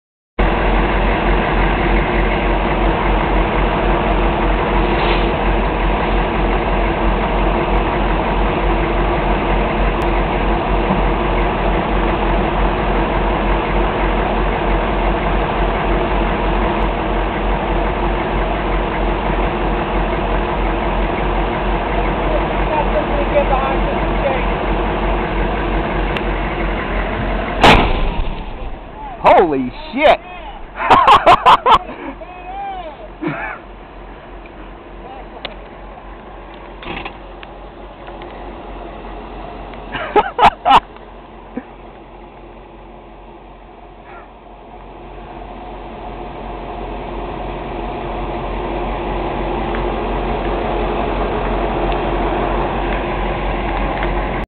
Iraq 04 05 Unexploded Ordnance demolition.
Army ops wait for it, big bang.